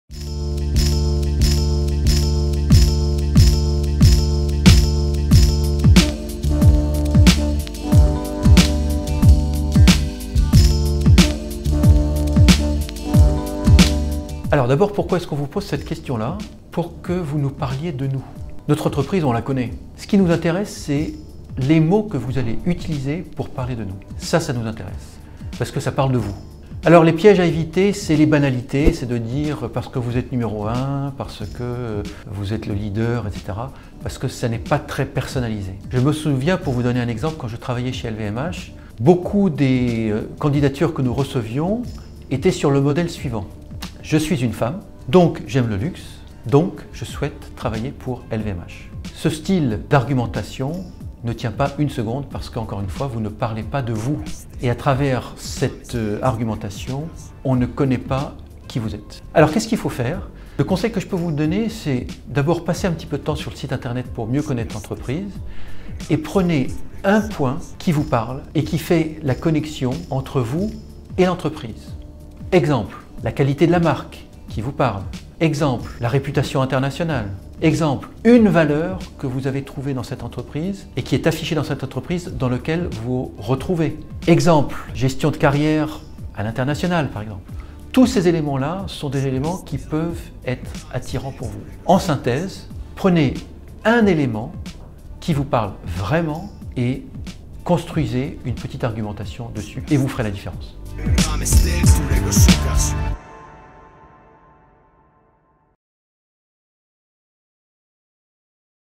Réussir son entretien d'embauche: répondre à "Pourquoi avez vous postulé chez nous?"
Marc Beretta donne quelques astuces pour répondre mieux à cette question qui peut paraître "piège".